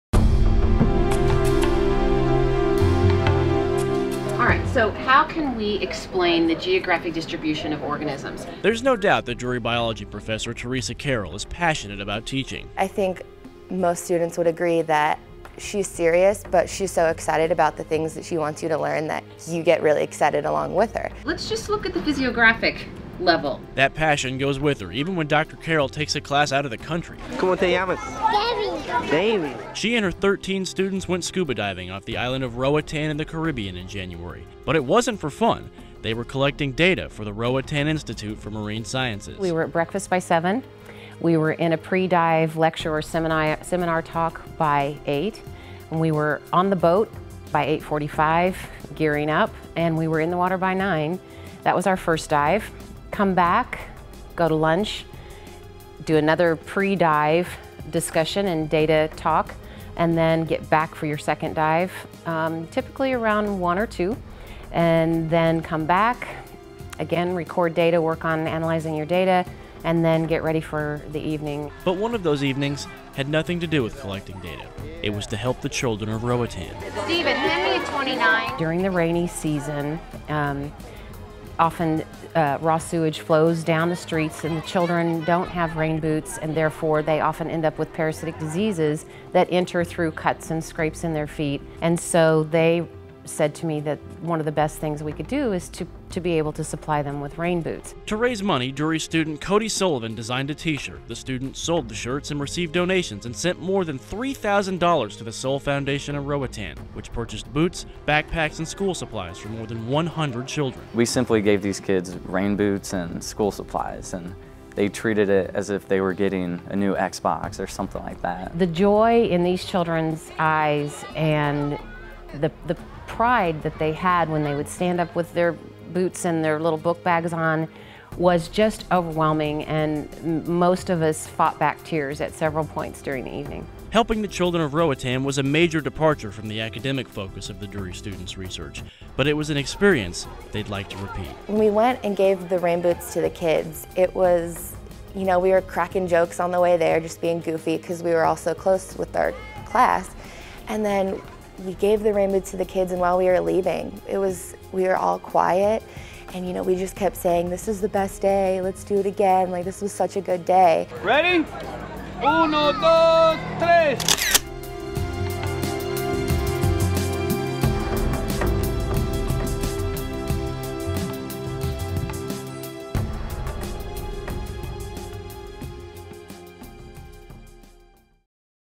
Rainboots for Roatan - Study Abroad Service at Drury University
In January 2013, Drury University's Dr. Teresa Carroll led a group of students on a research study abroad trip to Roatan. Prior to that trip, students raised money to purchase rainboots for the children of Roatan, which they distributed while there.